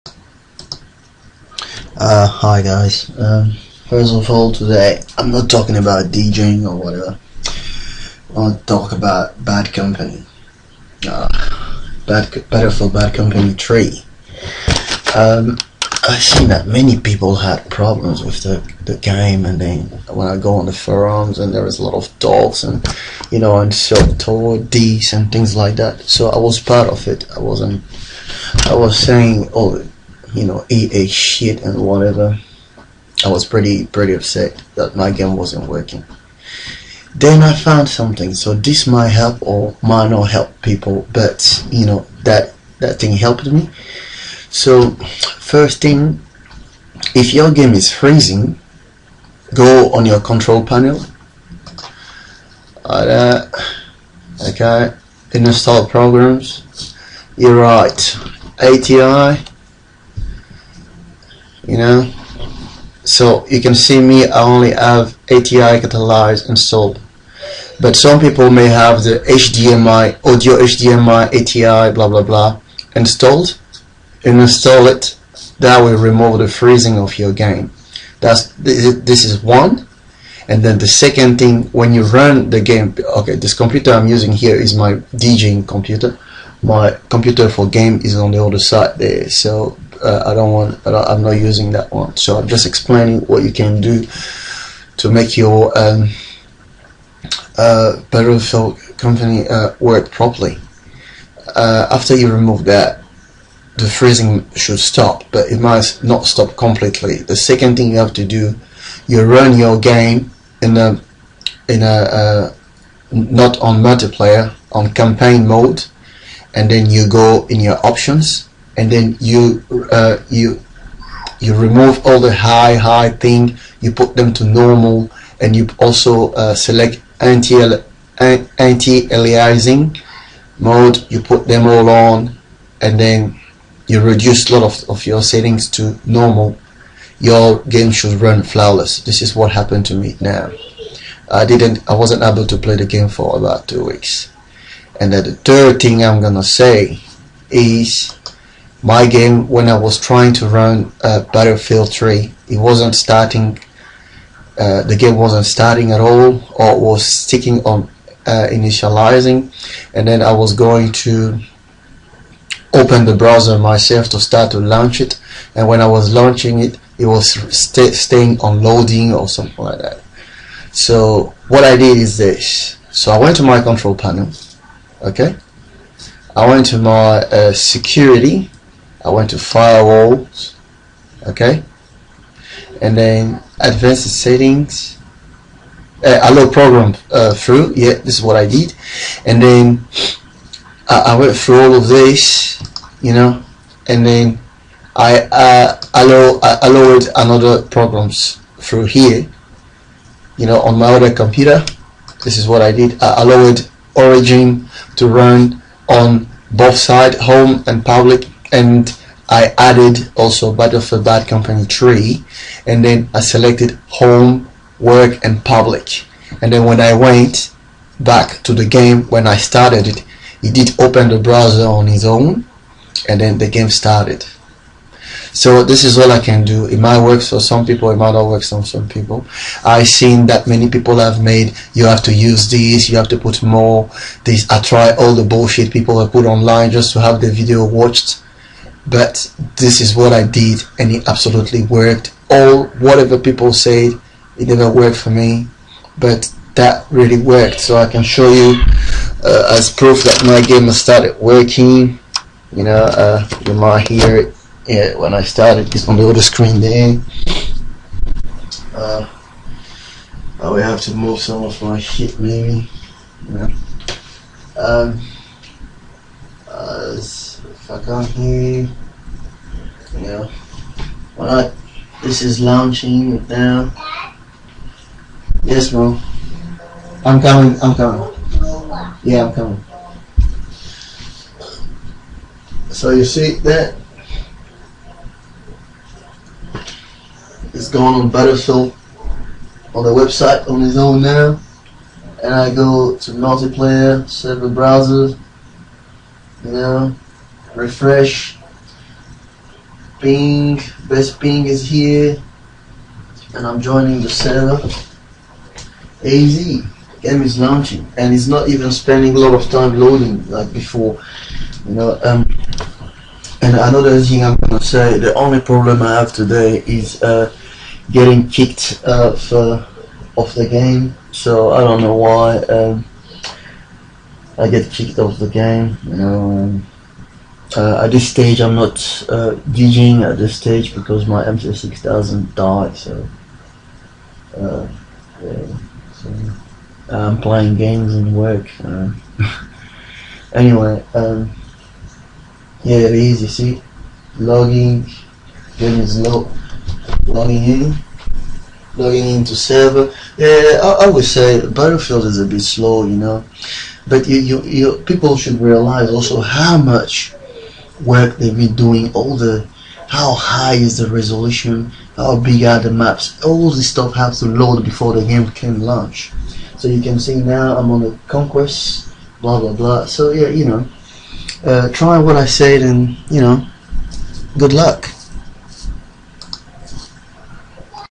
how to fix issues with battlefield 3, bug crashes
this is a very simple way to fix battlefield 3 and make it work for you.